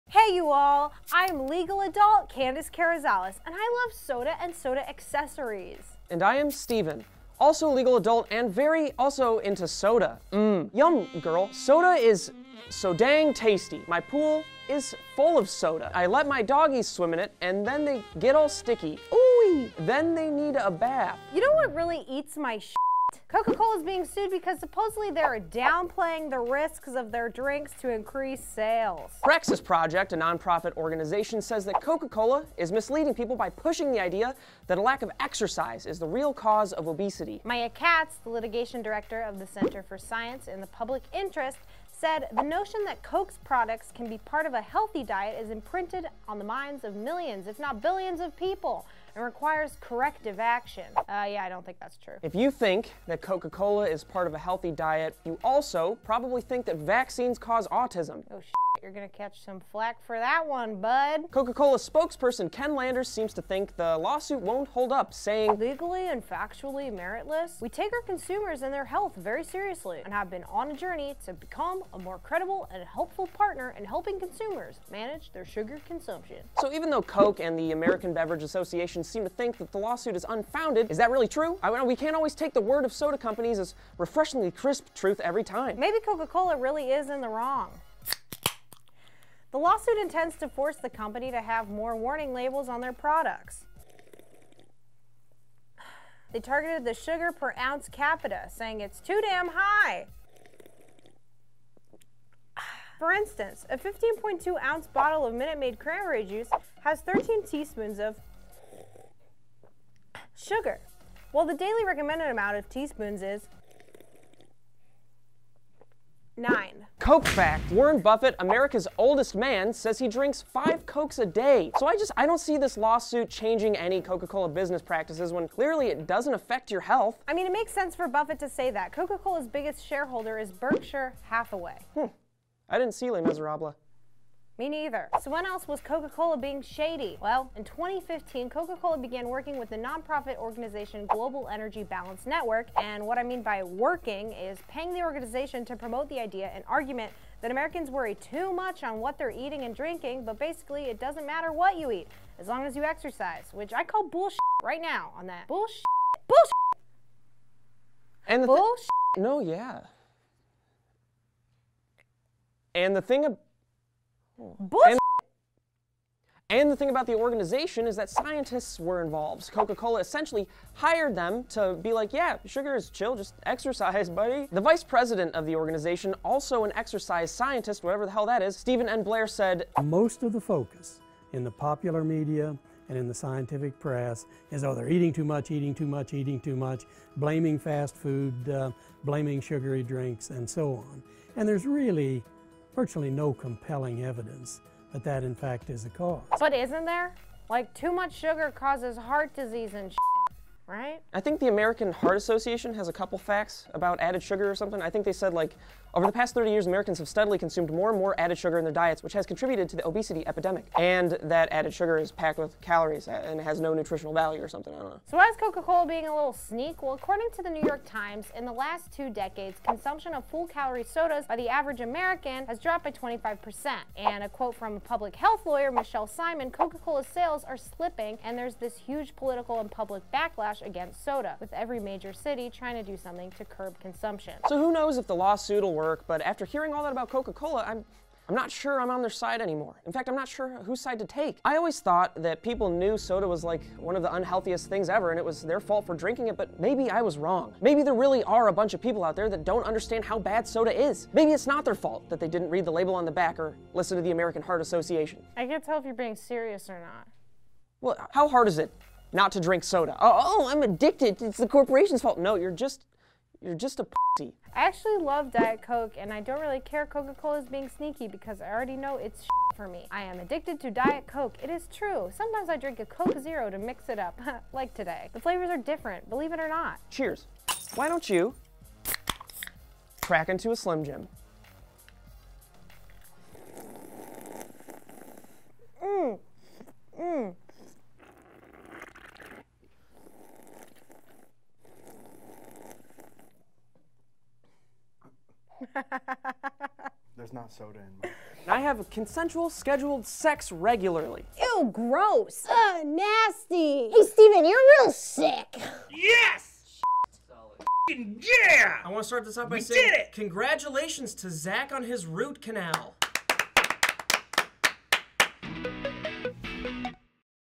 Coca-Cola Says You’re Fat and It’s Your Fault
There’s a lawsuit against soda star “Coca-Cola” saying they downplay the dangers of their sugary drink. But what scientists won’t tell you, is that you don’t have to drink it.

Edited by:
Danny Conner